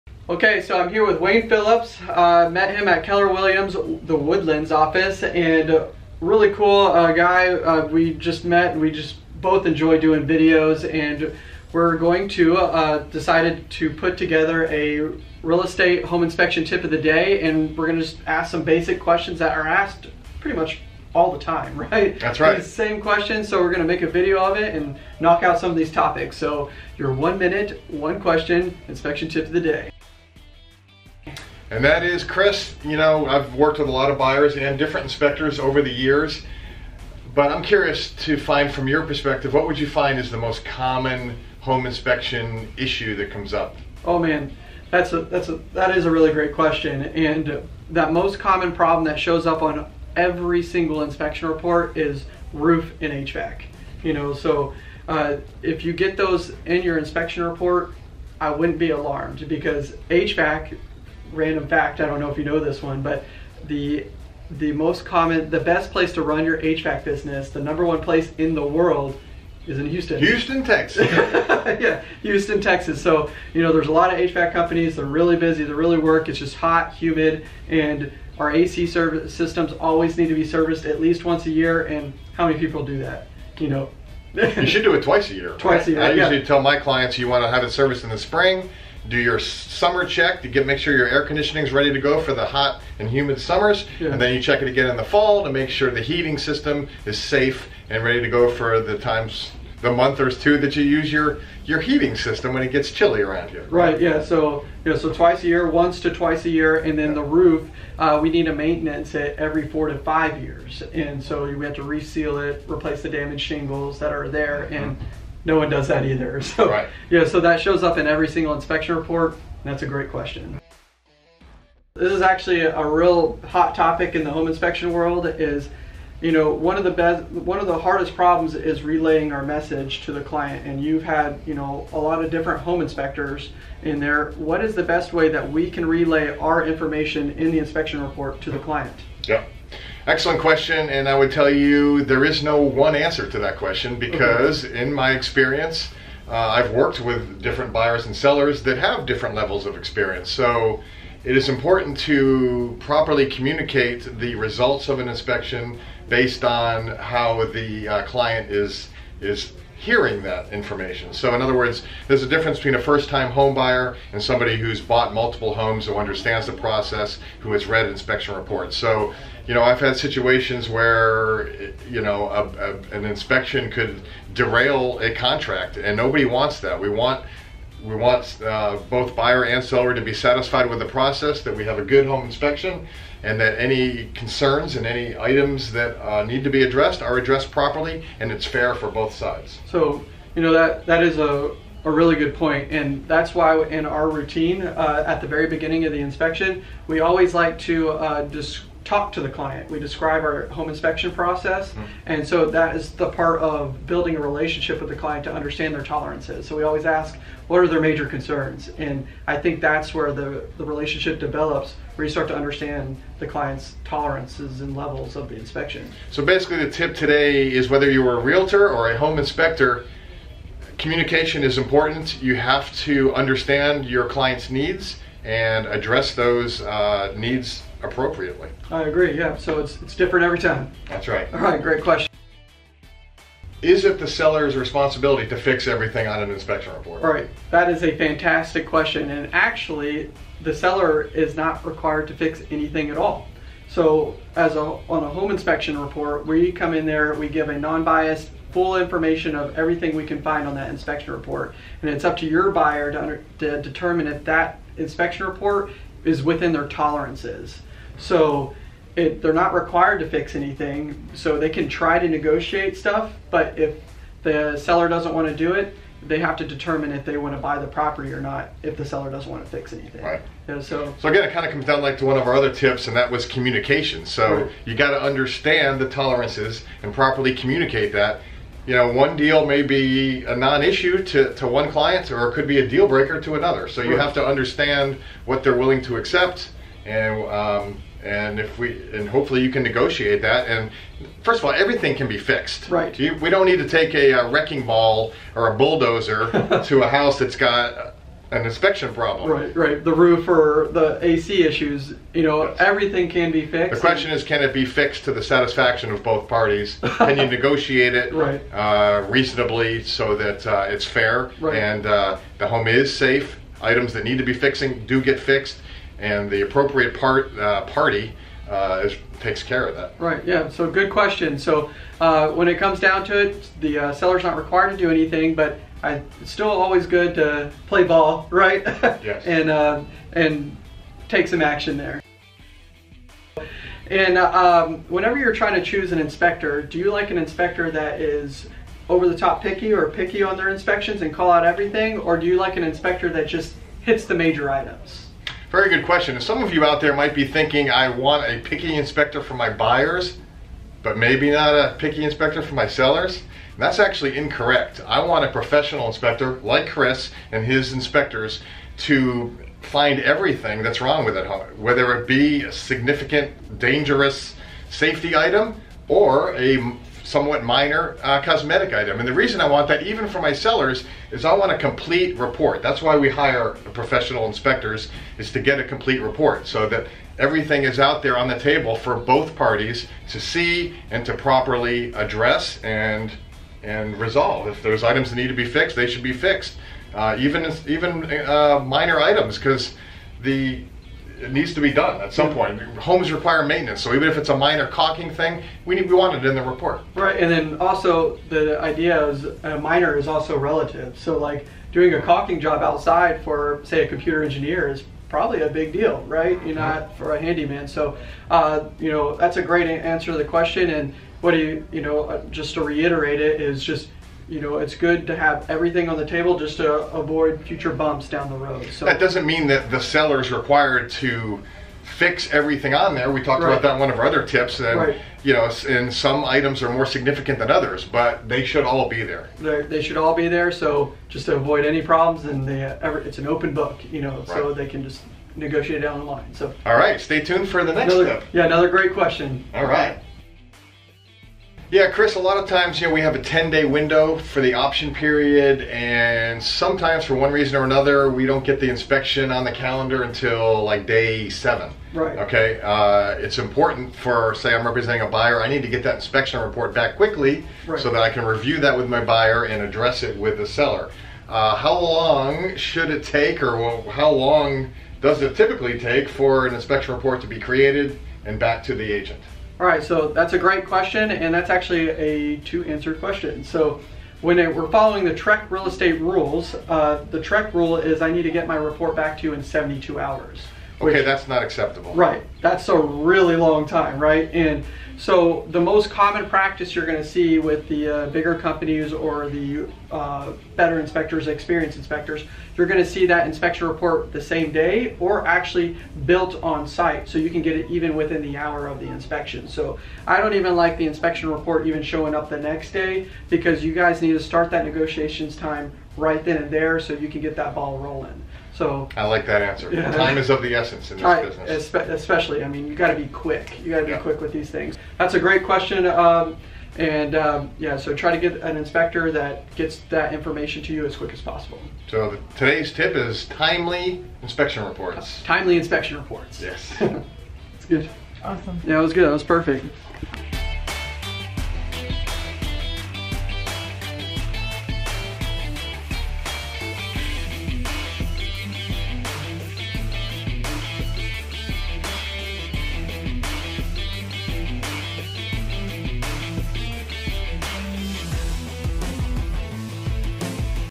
Real Estate/Home Inspection Tips w/ Wayne Phillips - The Houston Home Inspector
This week we went up to Keller Williams - The Woodlands to meet up with one of their real estate agents Wayne Phillips. We discussed several questions related to home inspections and purchasing a home. Let's go check it out!

A-ActionHouston.com
HoustonTexasHomeInspection.com

Next Youtube video out come and check it out.